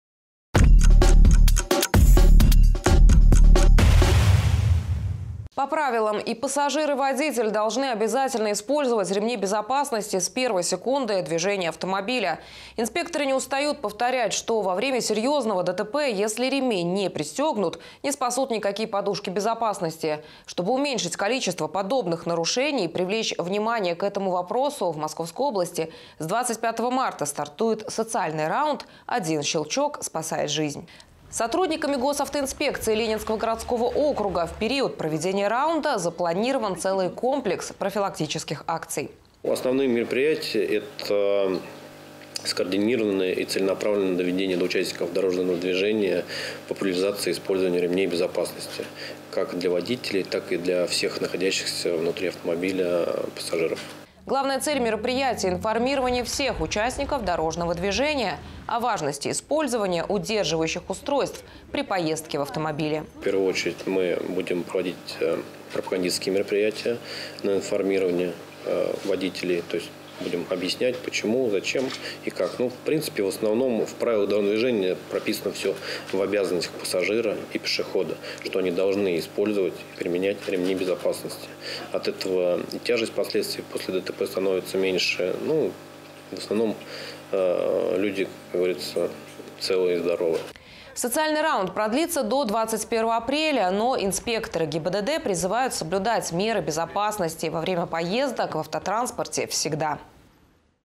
СОЦИАЛЬНЫЙ РАУНД
По правилам и пассажир, и водитель должны обязательно использовать ремни безопасности с первой секунды движения автомобиля. Инспекторы не устают повторять, что во время серьезного ДТП, если ремень не пристегнут, не спасут никакие подушки безопасности. Чтобы уменьшить количество подобных нарушений и привлечь внимание к этой проблеме в Московской области с 25 марта стартует социальный раунд «Один щелчок спасает жизнь».